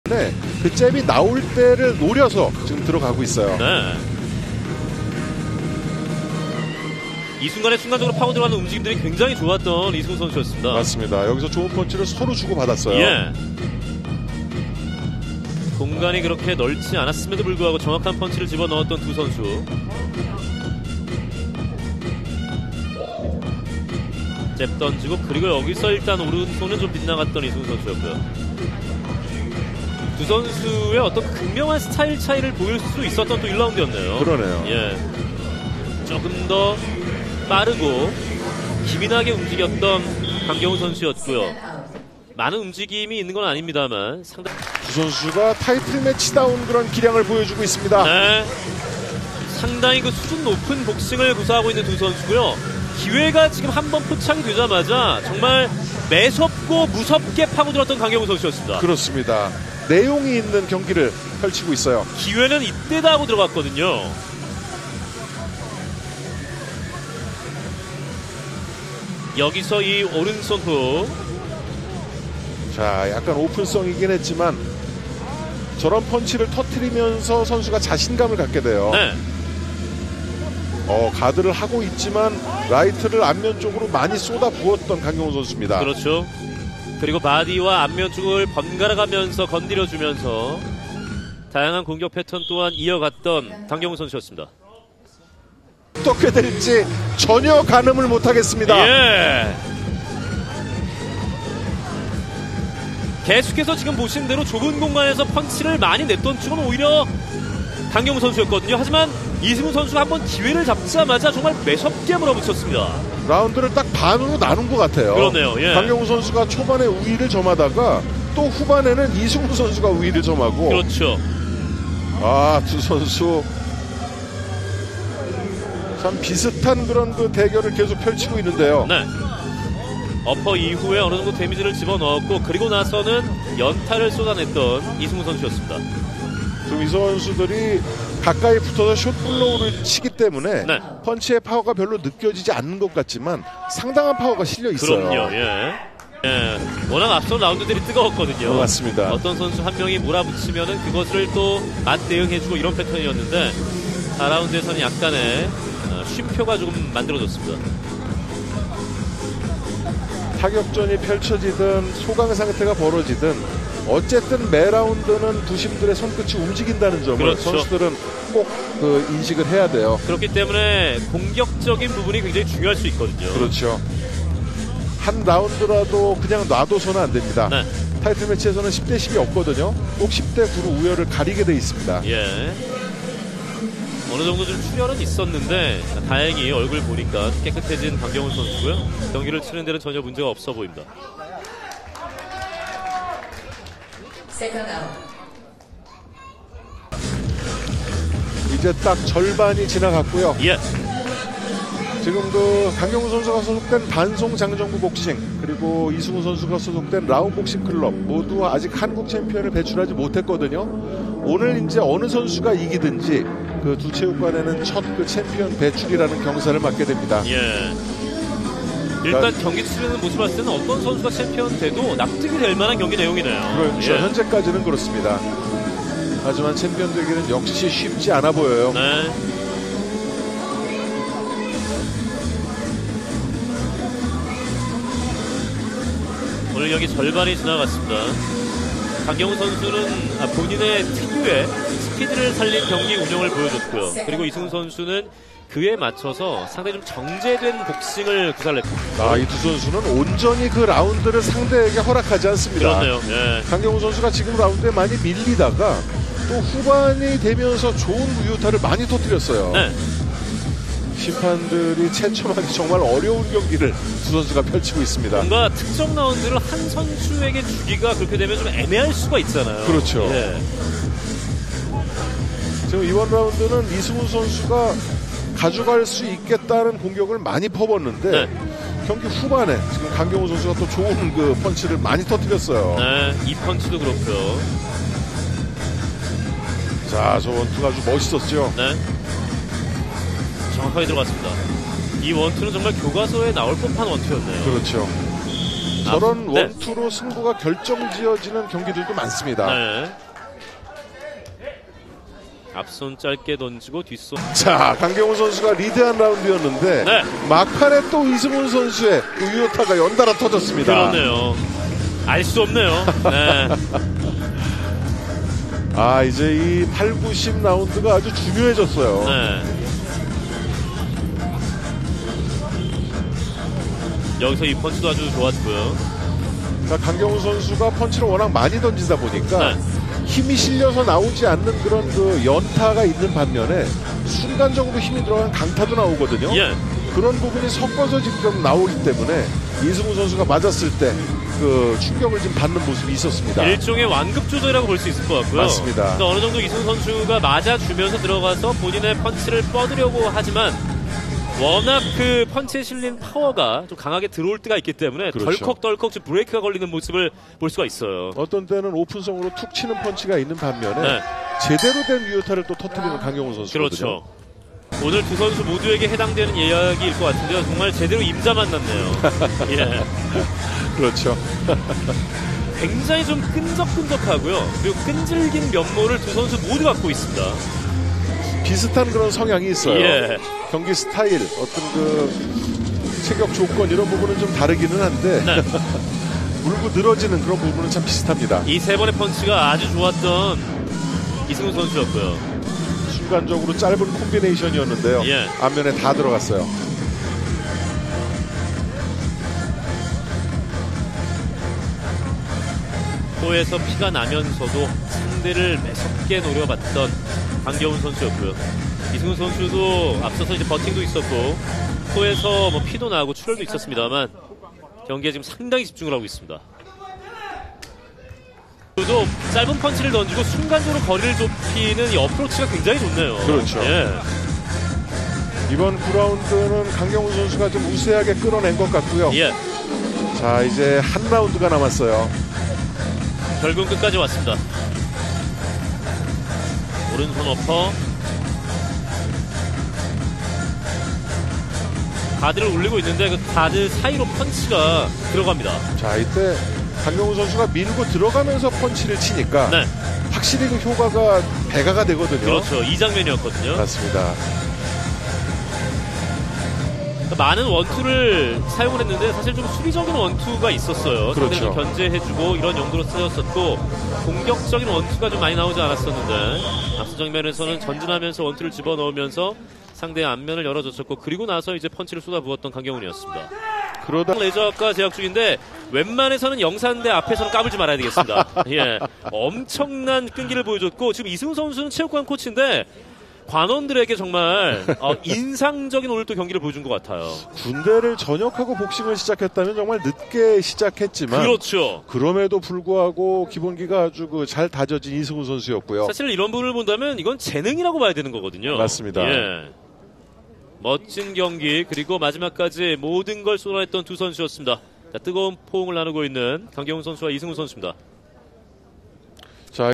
복싱m 한국 라이트급 타이틀 매치[하일라이트) 강경훈(반송 장정구) vs 이승훈(대구 라온)
한국 라이트급 타이틀 전 tvn sport 방송 하일라이트 강경훈 vs 이승훈